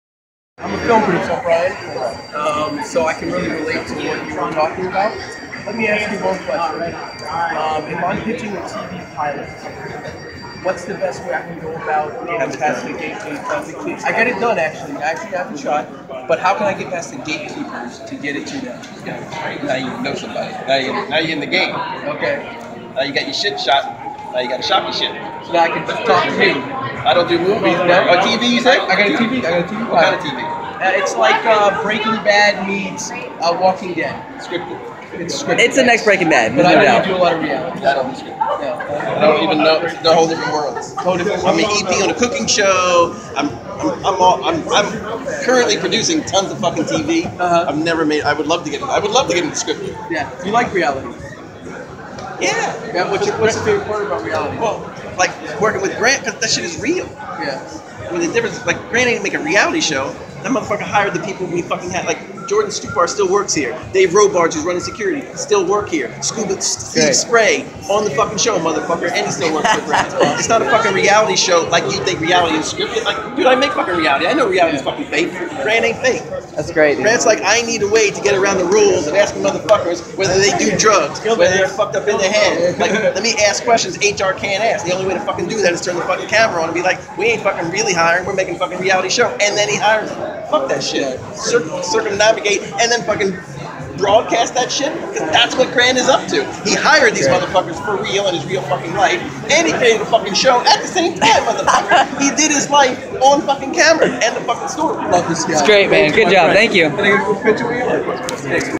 Grant Cardone's 10X GrowthCon in Miami with Hank Norman (Jalbert Brothers)
Jalbert Brothers With Hank Norman at Grant Cardone's 10X GrowthCon in Miami

How to Shop you Film or TV Pilot! Filmmaking Tips

FILMS
House Guest
10 seconds to run